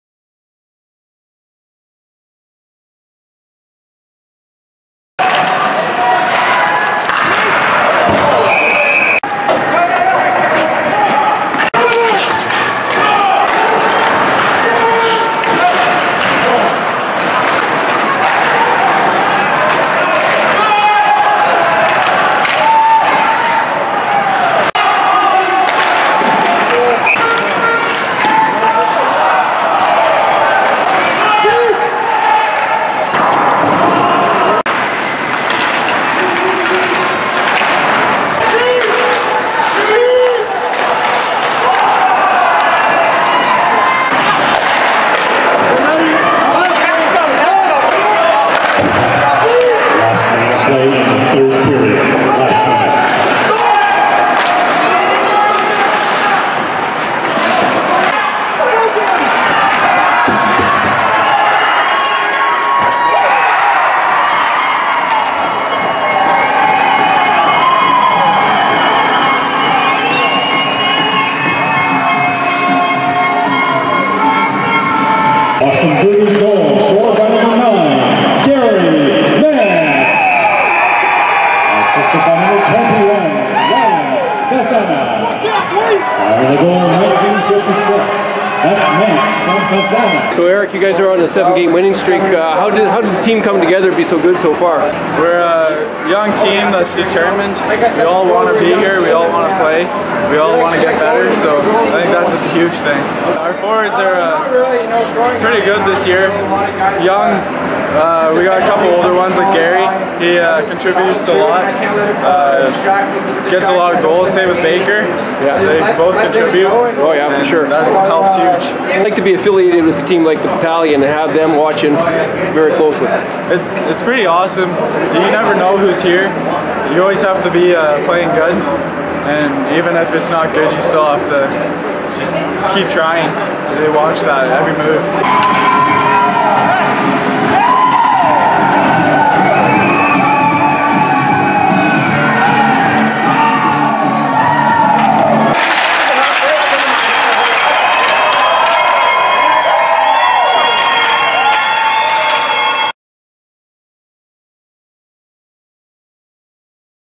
Powassan Voodoos Oct 30, 2016
Powassan Voodoos of the NOJHL are ranked 11th in Canada after pushing their winning streak to seven beating Iroquois Falls 4-2, Oct. 30, 2016. Gary Mantz scored a hat trick, his 13th goal giving him 29 points to lead his team offensively, third in the league. Voodoos lead the NOJHL with a 14-3-1 record, a point above Cochrane in the East Division. Mantz and Dayton Murray were named Eastlink TV stars for their play last week, Murray leading blueliners with 8 goals.